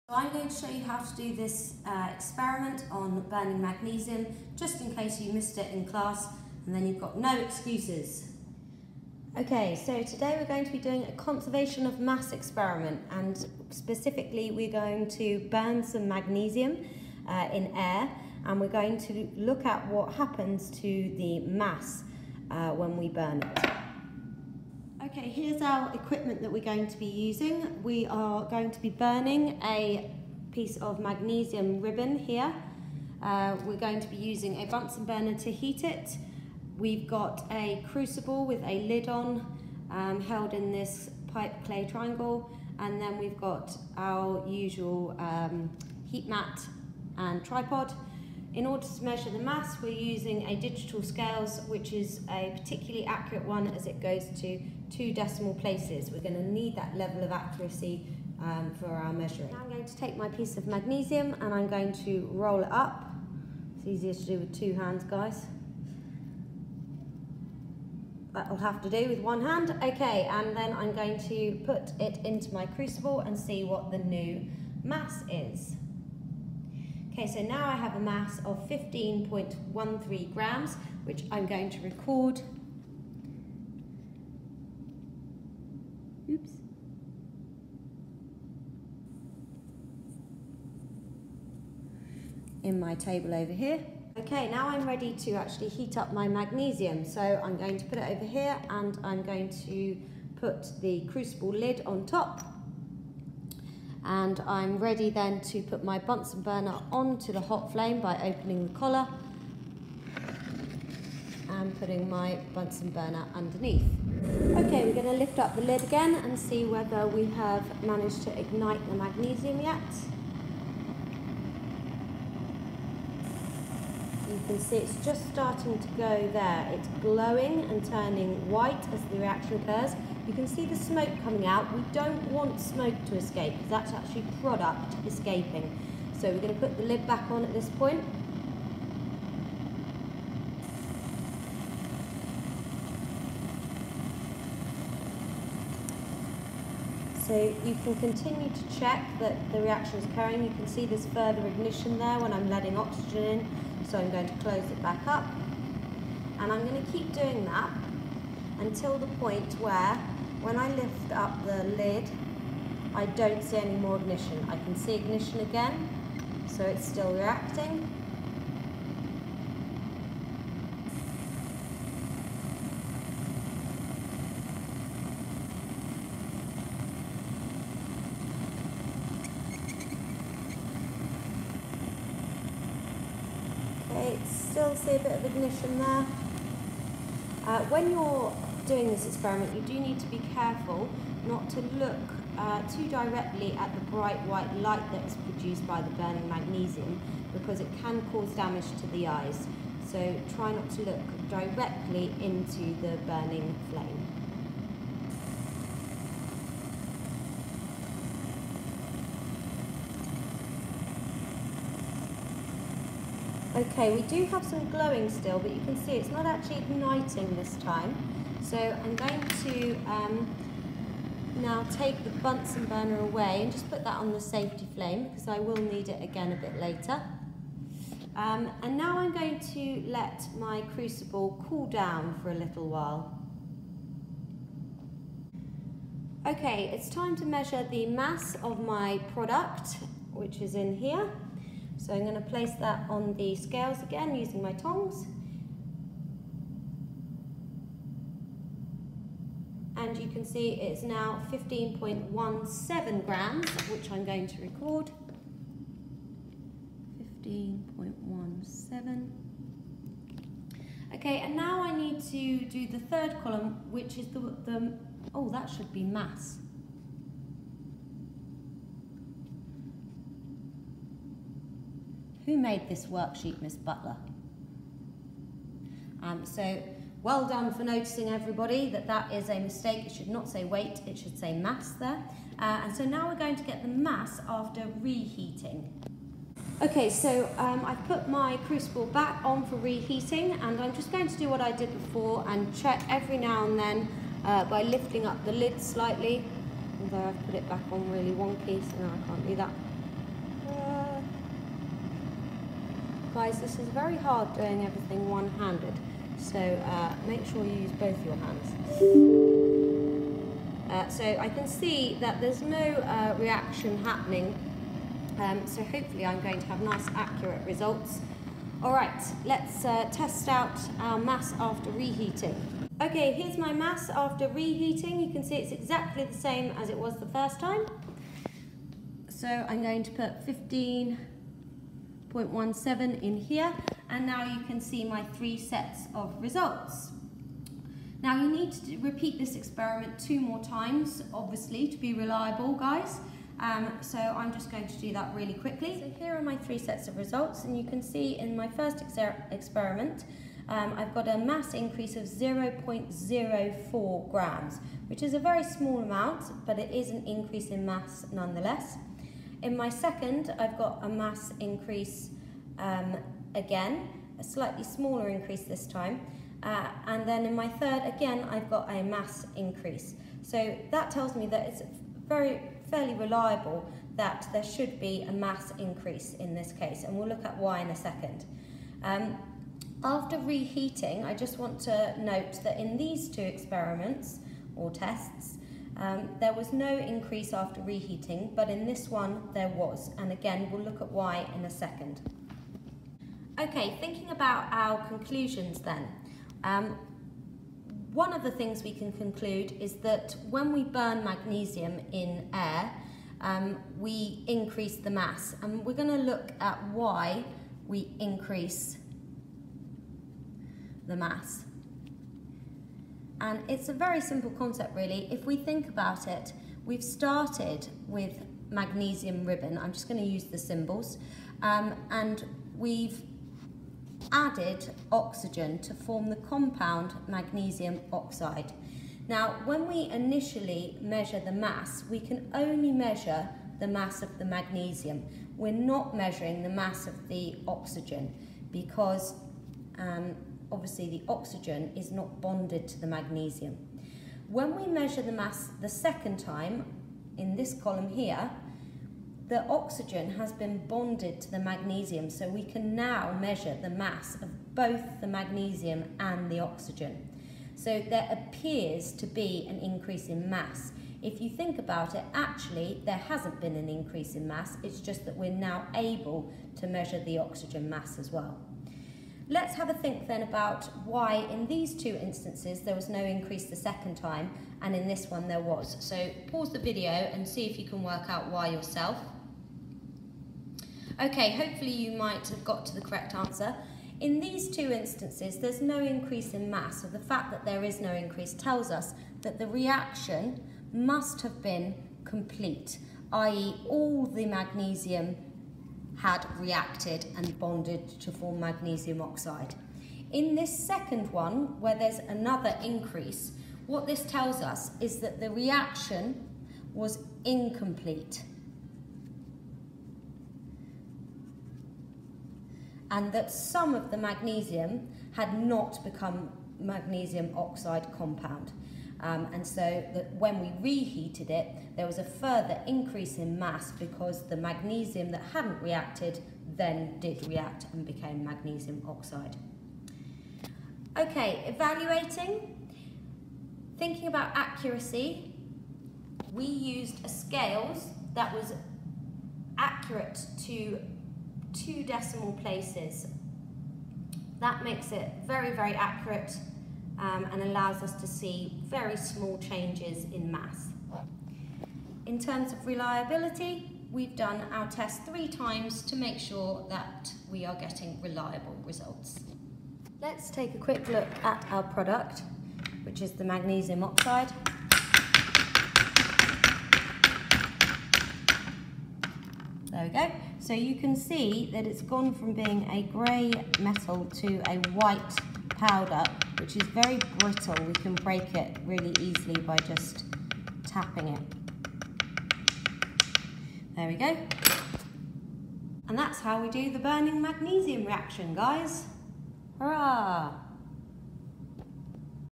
MAGNESIUM & CONSERVATION OF MASS
Heating magnesium strongly in air to create the compound magnesium oxide.